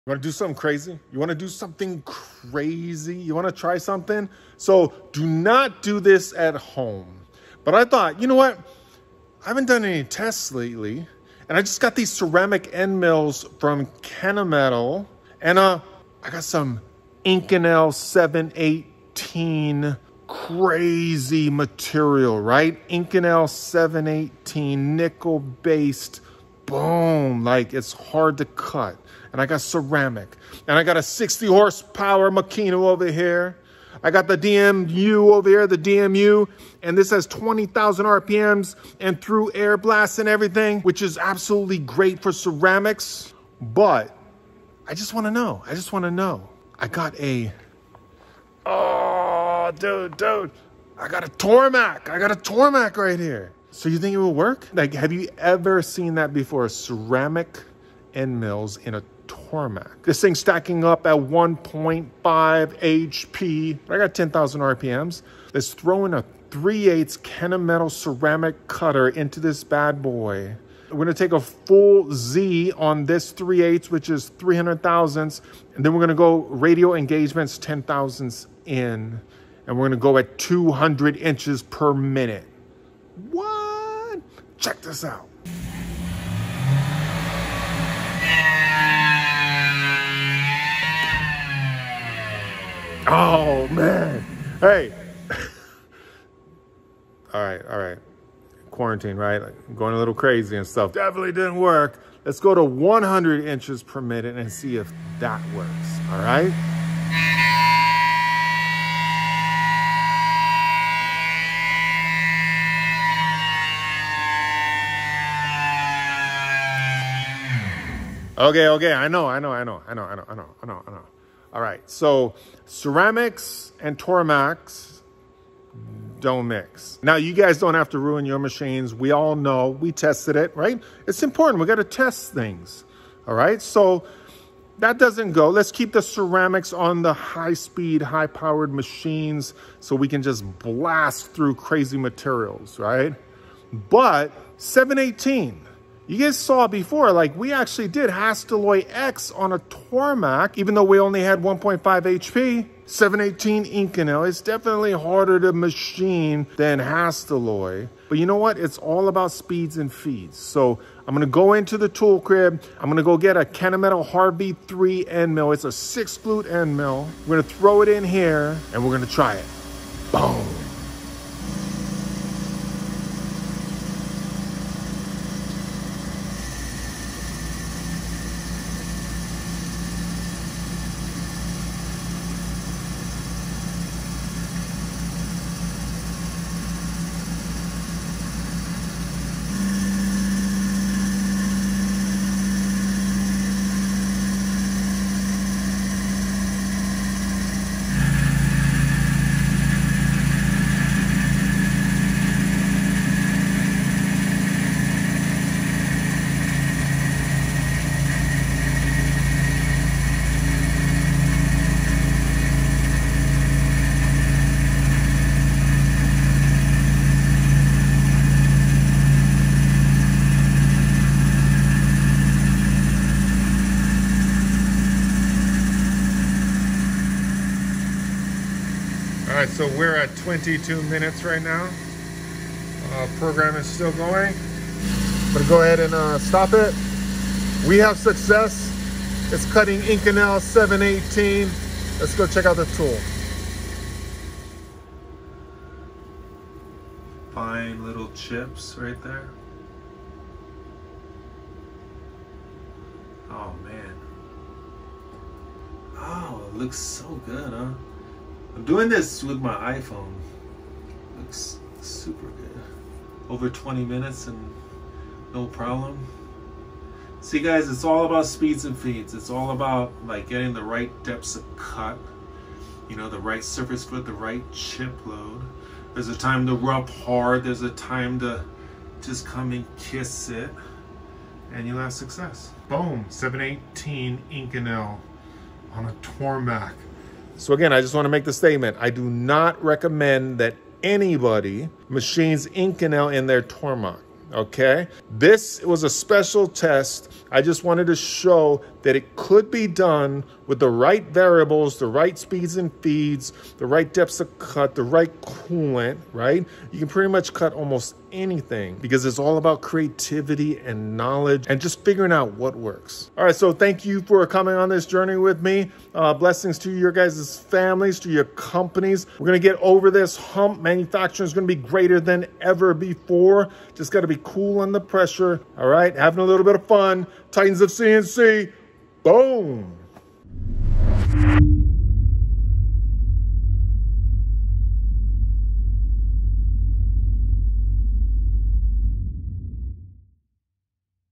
IMPOSSIBLE MACHINING TEST: Inconel 718, Kennametal Ceramic End Mills & 1.5 HP TORMACH
MB017E7JAFBH4VC